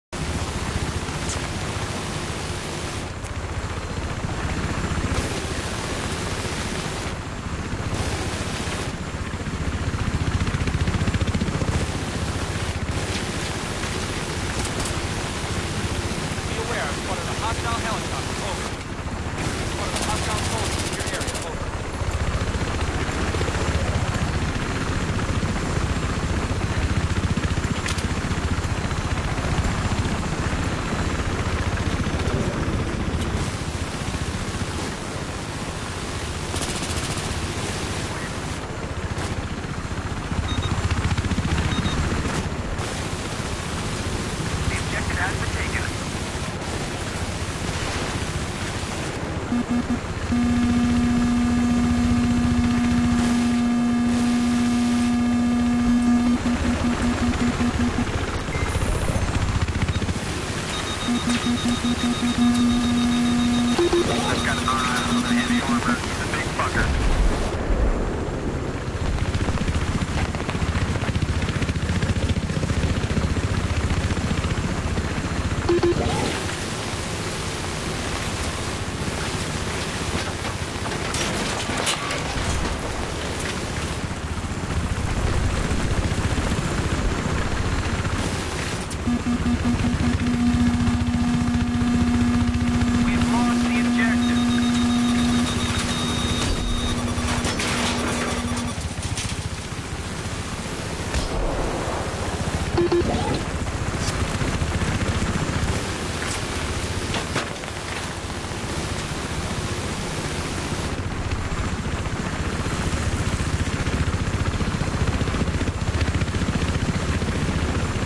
chop 2 chop jav
triple kill with a jav from one chopper to another. Enjoy